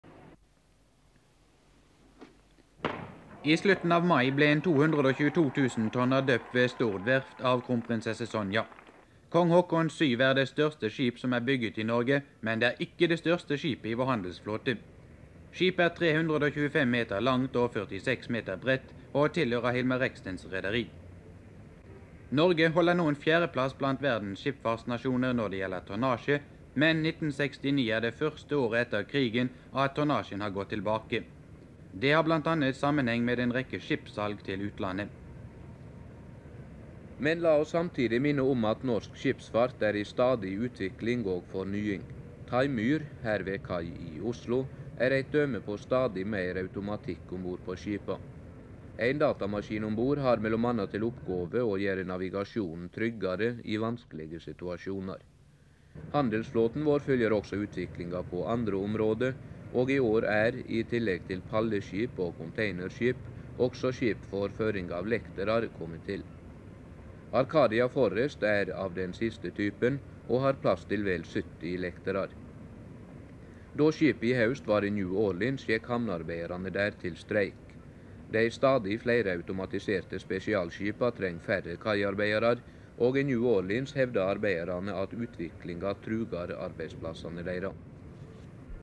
Handelsflåten i 1969
Vi ser b.la. VLCC Kong Haakon VII, M/S Taimyr og Acadia Forest.